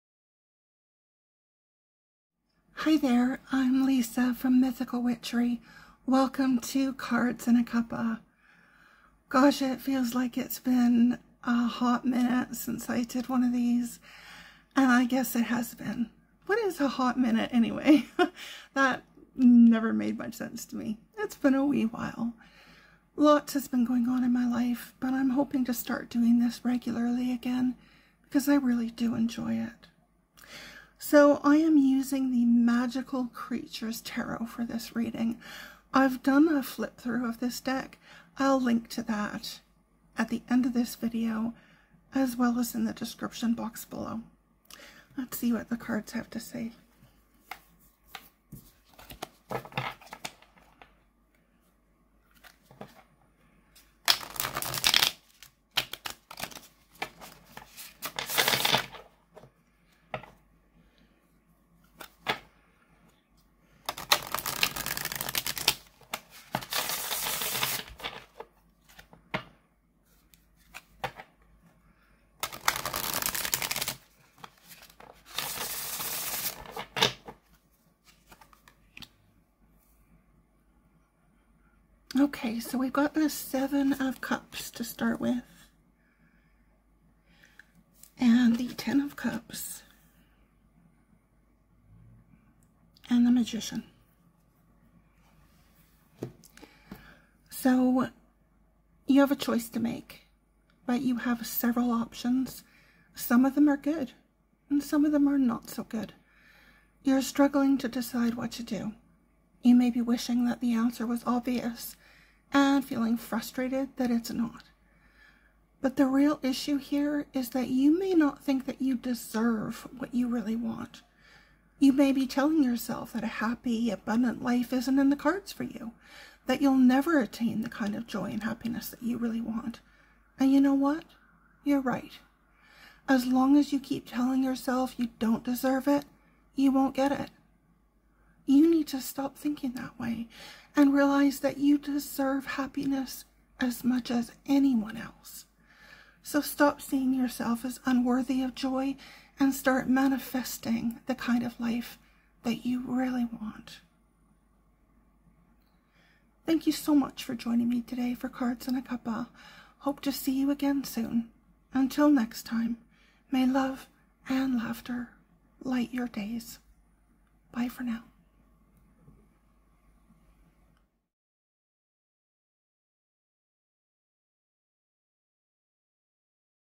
Collective Tarot Reading - 235
💜 Collective Reading 235 - Seven of Cups, Ten of Cups, The Magician💜
I am using the Magical Creatures Tarot for this reading. If you'd like to see a full walkthrough of this deck, the link is